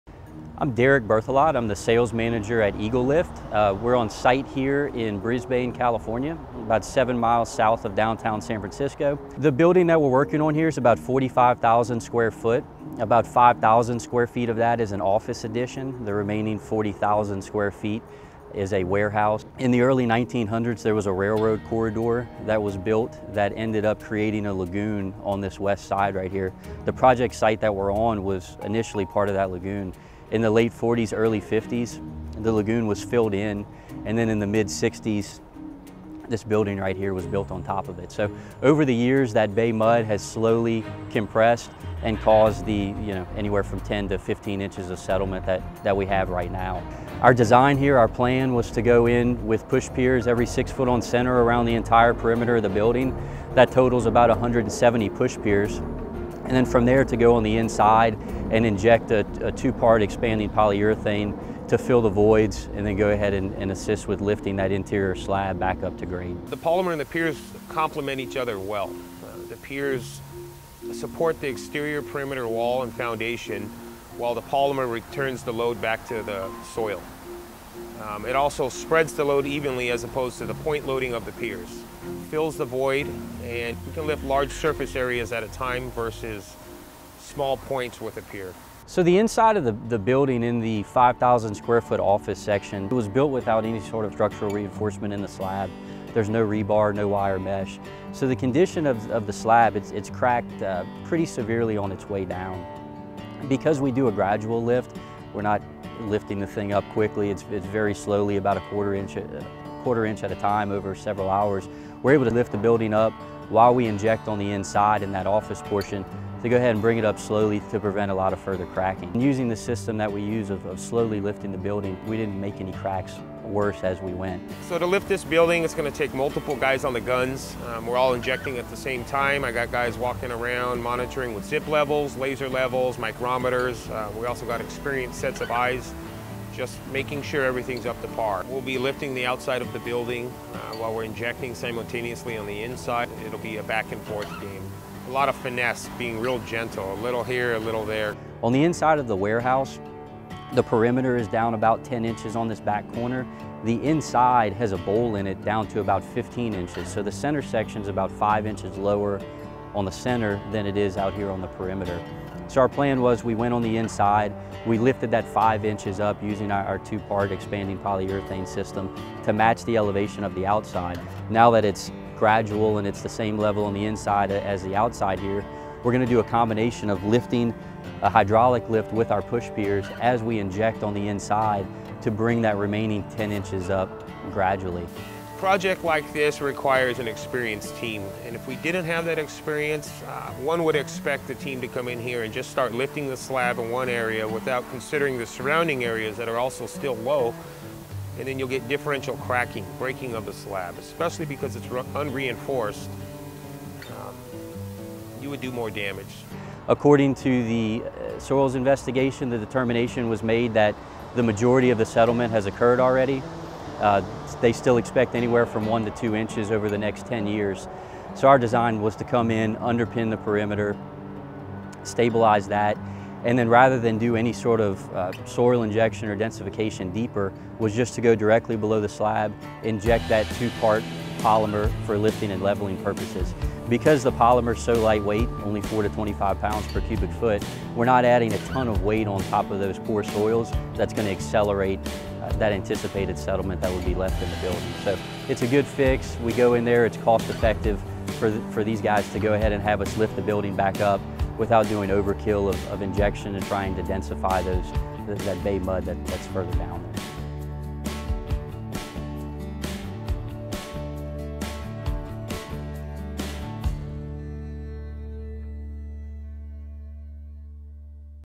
15 inch Lift of a 45,000 sqft Warehouse
EagleLIFT completes an incredible project in Brisbane, CA, where we lift a 45,000 sqft warehouse 15 inches from exterior to interior. The project is completed using a combination of Push Piers and Polyurethane Grout.